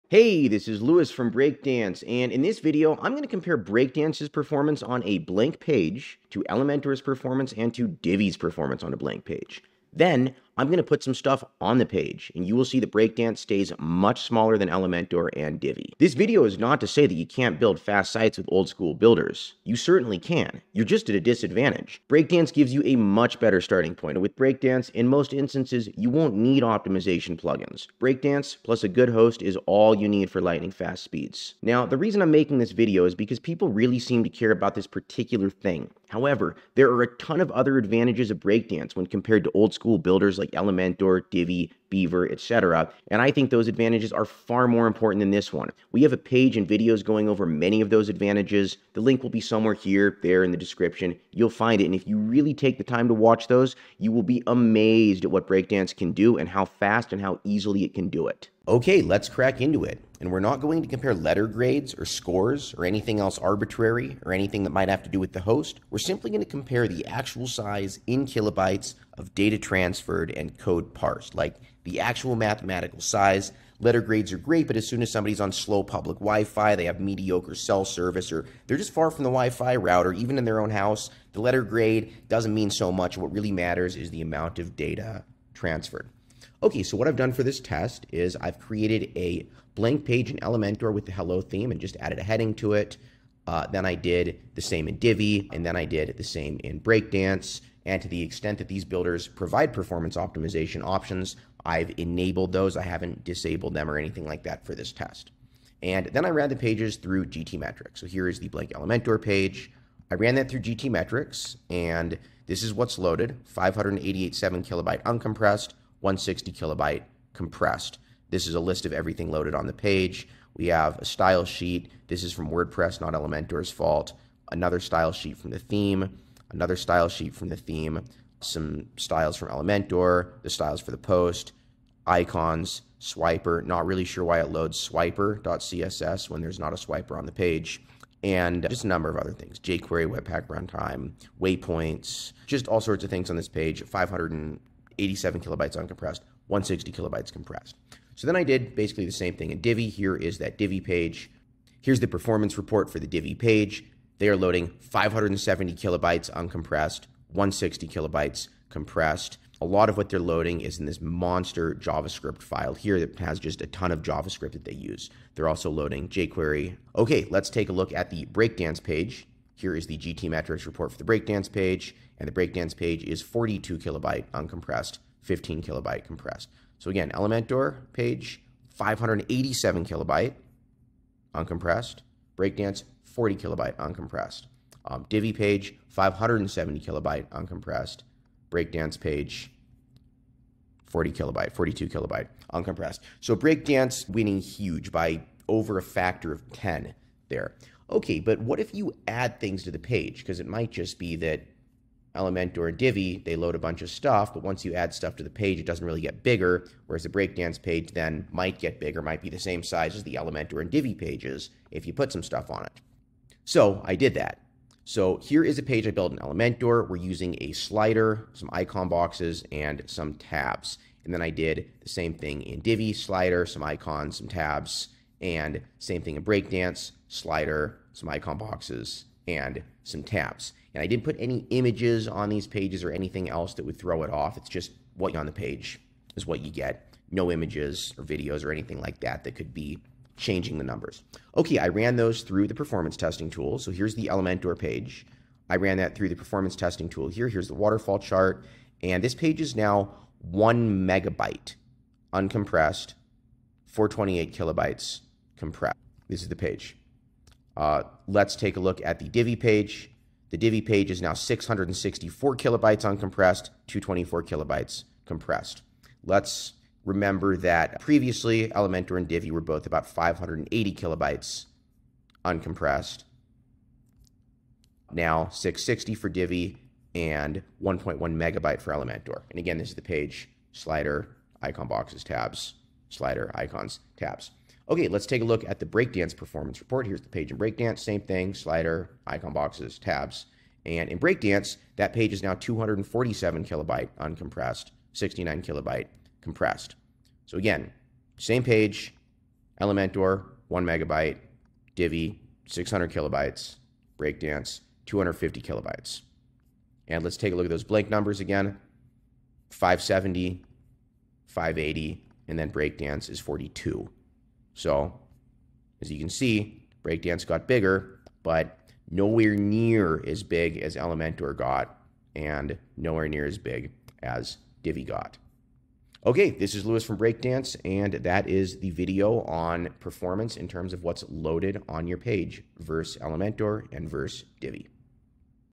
Breakdance vs Elementor / Divi - Performance & Bloat
We compare the performance and bloat between Breakdance and Elementor / Divi.